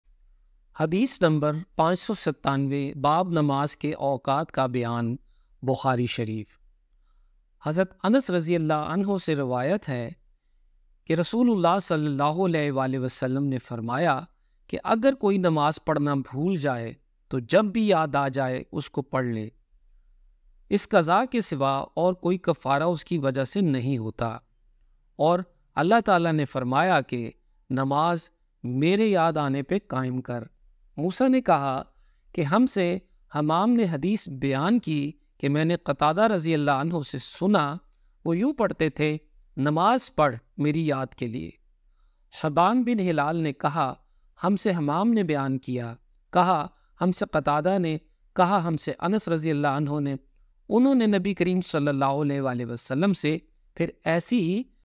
Hadith No 597 Sahih Bukhari Chapter Namaz K Auqat ka Byan|Urdu|Hindi with English subtitlesحدیث نمبر
S.K.S (Syed Kashif Safdar)

Hadith No 597  Sahih Bukhari Chapter Namaz Ka Bayan

Narrated Anas (RA): The Prophet ﷺ said, "If anyone forgets a prayer he should pray that prayer when he remembers it. There is no expiation except to pray the same." Then he recited: "Establish prayer for my (i.e. Allahs) remembrance." (20.14).